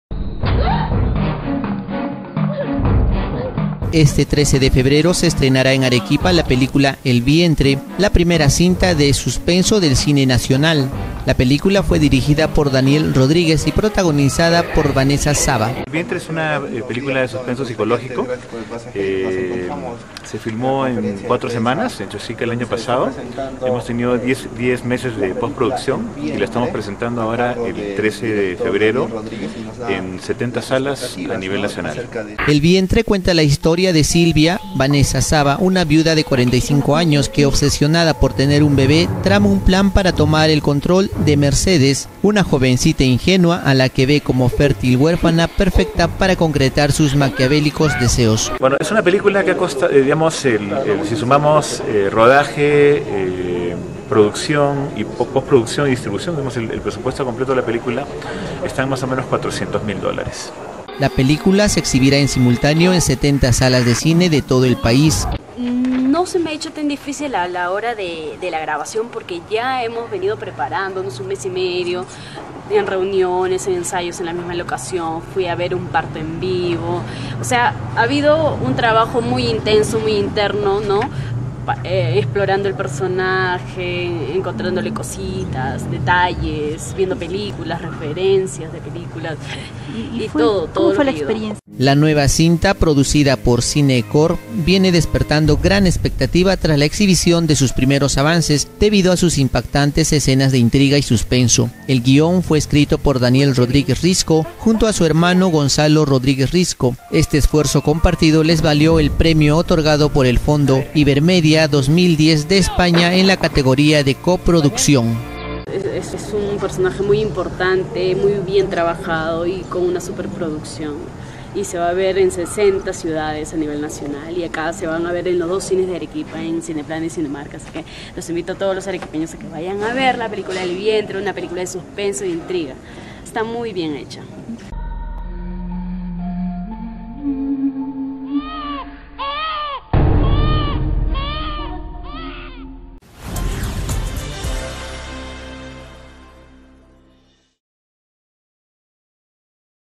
Este 13 de febrero se estrenará en Arequipa la película 'El Vientre'
Es la primera cinta de suspenso del cine nacional. La película fue dirigida por Daniel Rodríguez y protagonizada por Vanessa Saba.
El director explicó que 'El Vientre' cuenta la historia de Silvia (Vanessa Saba), una viuda de 45 años que, obsesionada con tener un bebé, trama un plan para tomar el control de Mercedes (Mayella Lloclla), una jovencita ingenua, a la que ve como fértil huérfana perfecta para concretar sus maquiavélicos deseos.